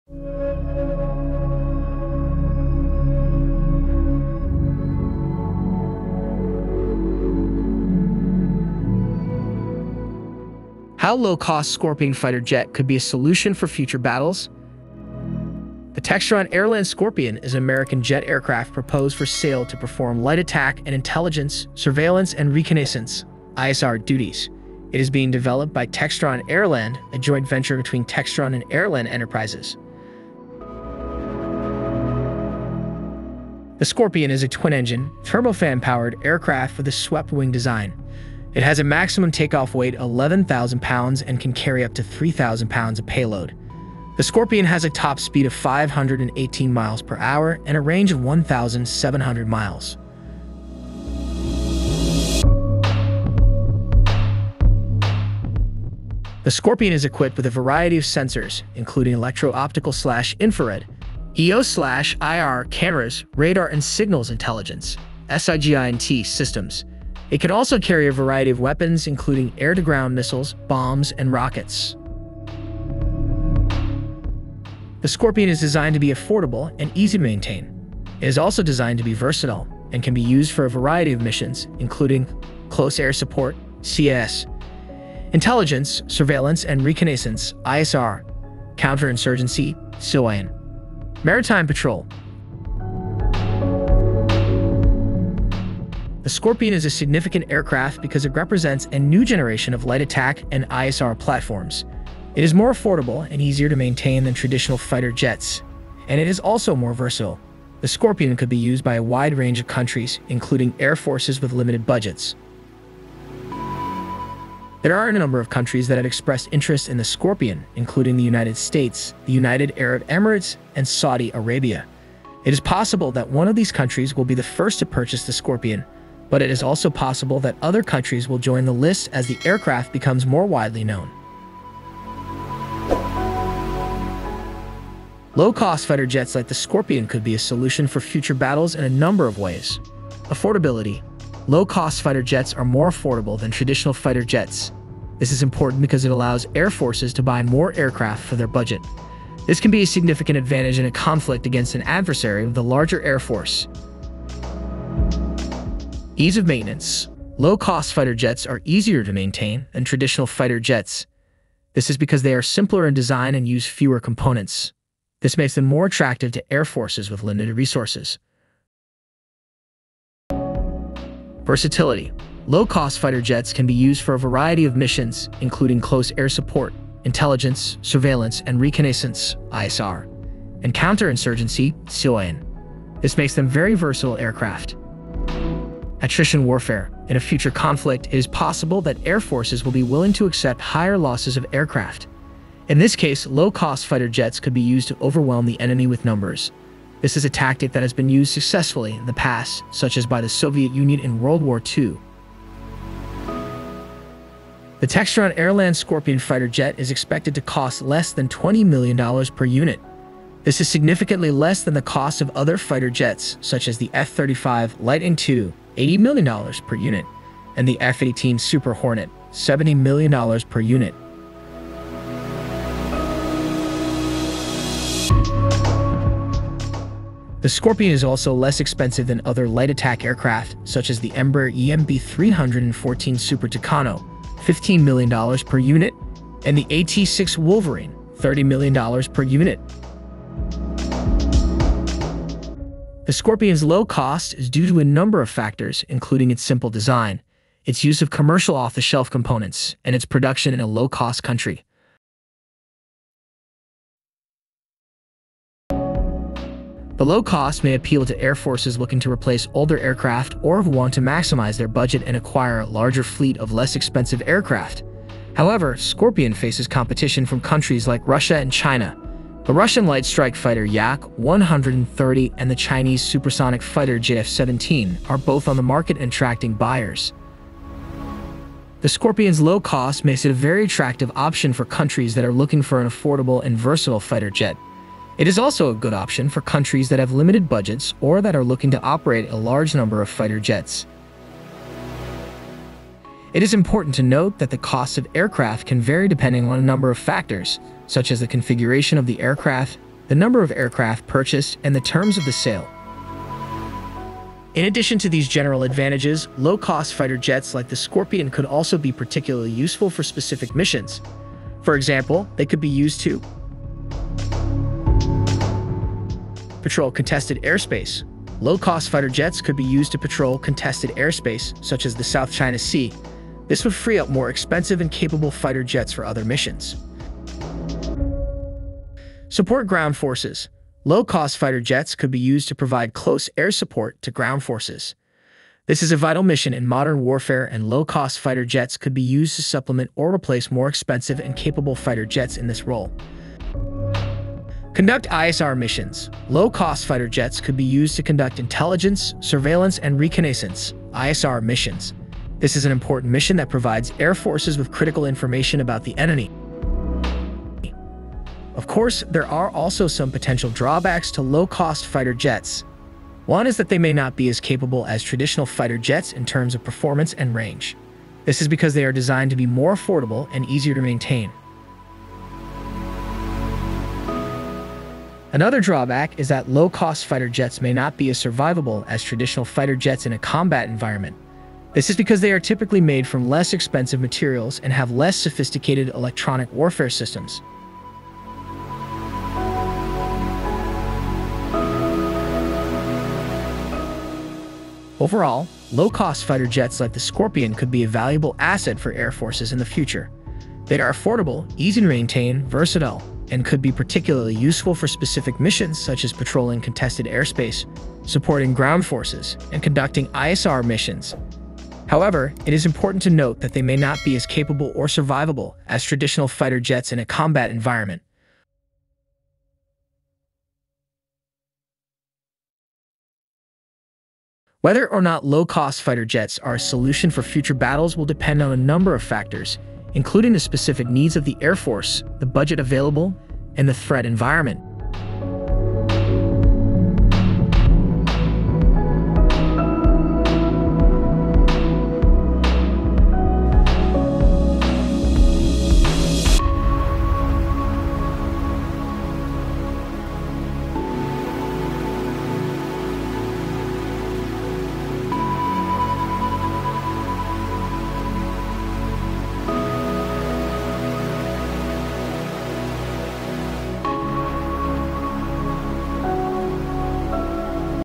Scorpion Fighter Jet: How Low Cost Fighter Jet Could Be a Solution For Future Battles?
The Textron AirLand Scorpion is an American jet aircraft proposed for sale to perform light attack and intelligence, surveillance and reconnaissance (ISR) duties. It is being developed by Textron AirLand, a joint venture between Textron and AirLand Enterprises.

All the Images and Video is not ours, it's just an illustration.